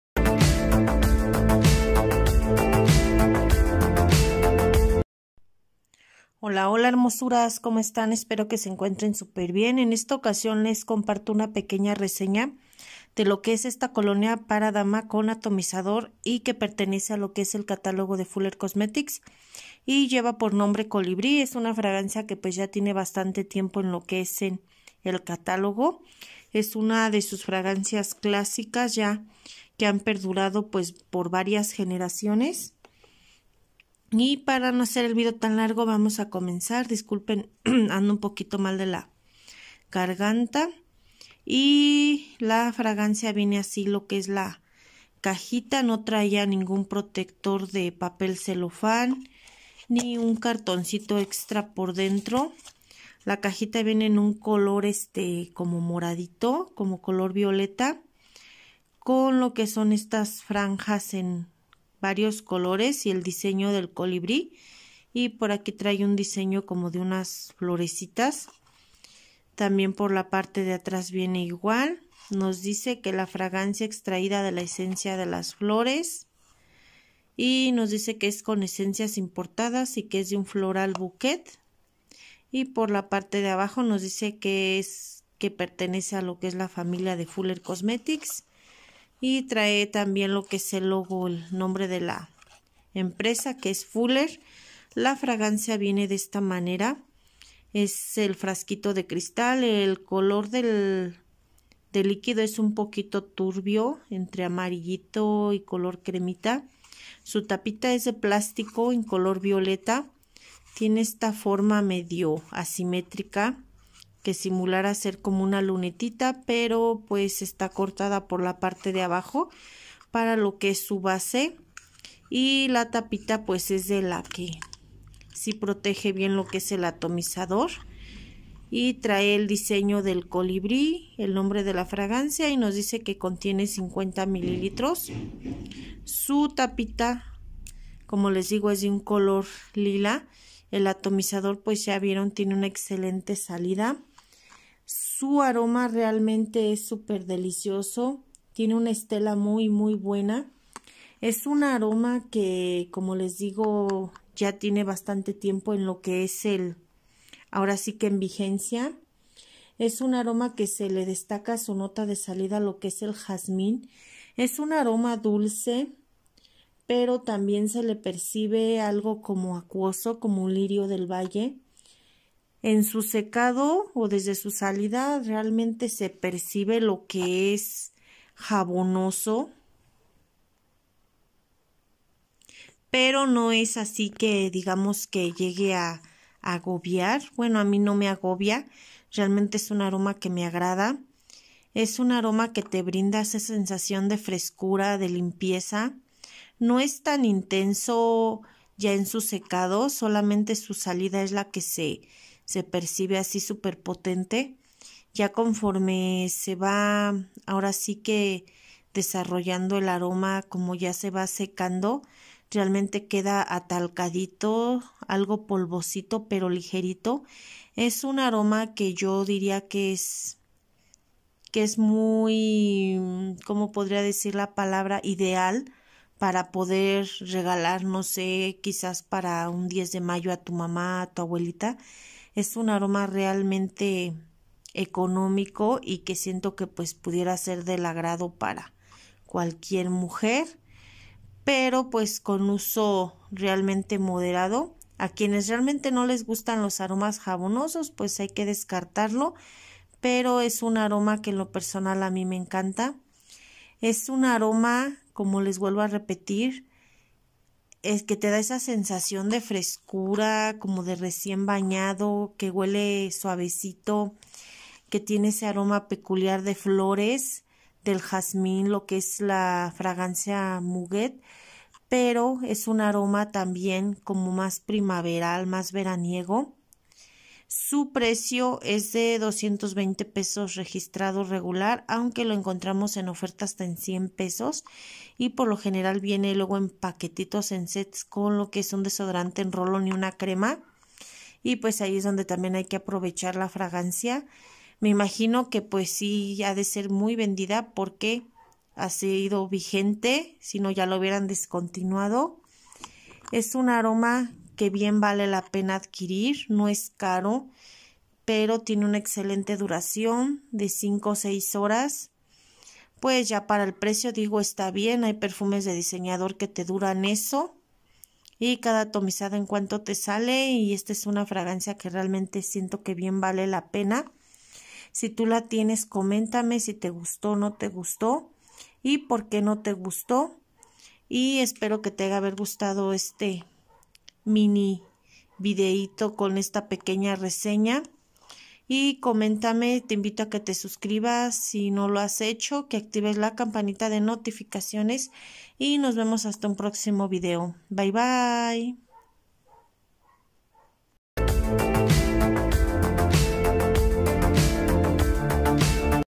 COLIBRÍ DE FULLER COSMETICS (MINI-RESEÑA)
Hola hermosuras les comparto una pequeña reseña de la colonia con atomizador para dama que se llama COLIBRÍ y que pertenece al catálogo de fuller cosmetics
Espero que sea de su agrado me dejen su dedito arriba y me comenten si la conocen, si les gusto, o no y porqué fue así?

Y más.